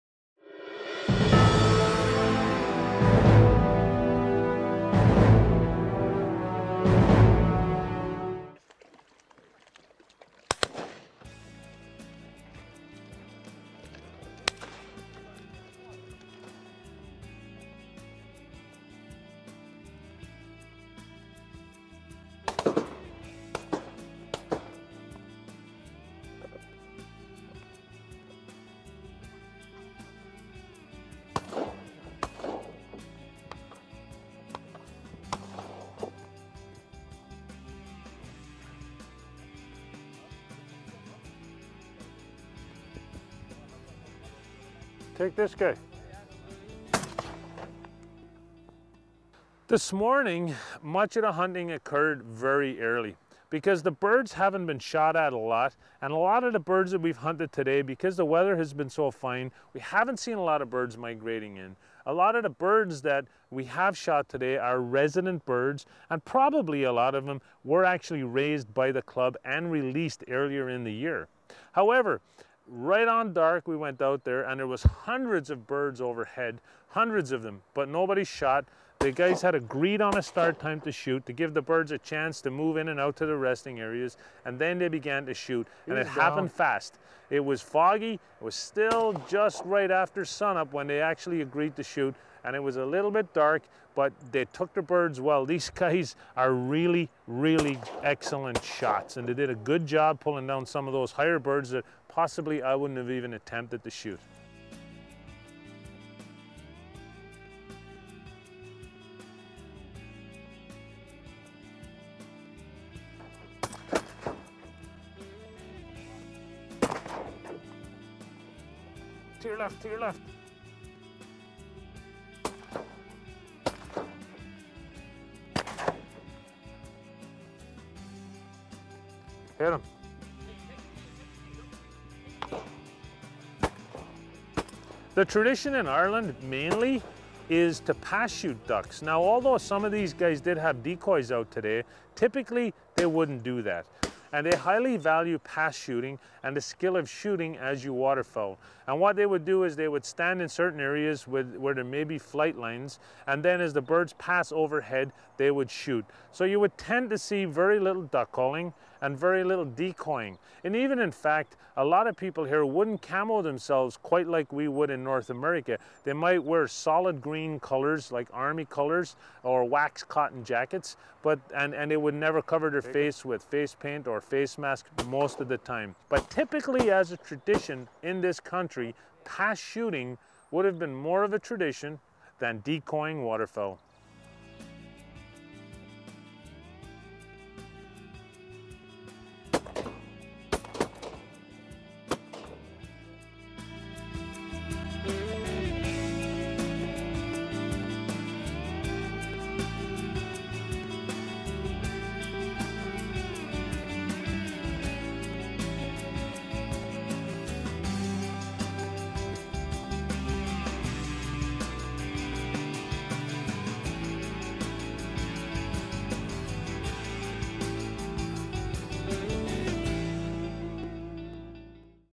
Duck Hunt: Ireland part 2
Duck hunting in Ireland again, some nice footage and excellent pass shooting. Shooting a small private club.